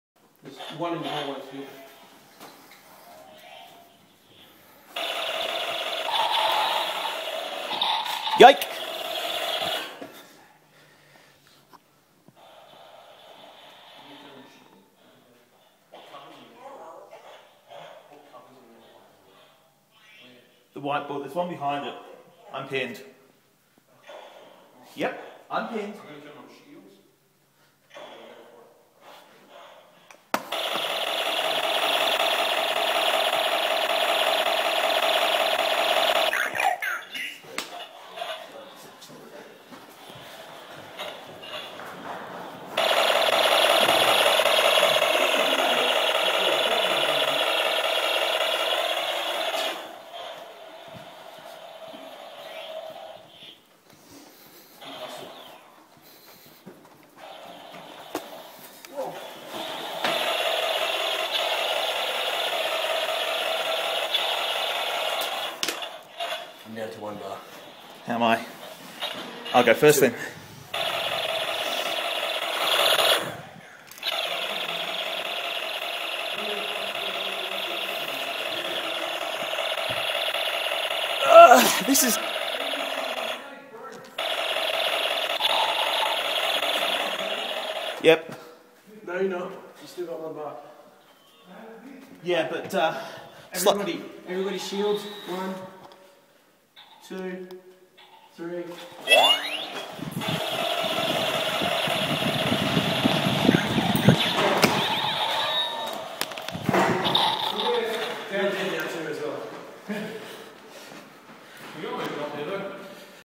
Light strike 3 v 2: ending on a shield rush
3 stock range blasters against two snipers. It ends with a shield rush which is like a less successful version of a Zerg rush.  All low on life, high on shields, and clean out of a better idea, we charge two snipers.

All things considered, it went better than expected.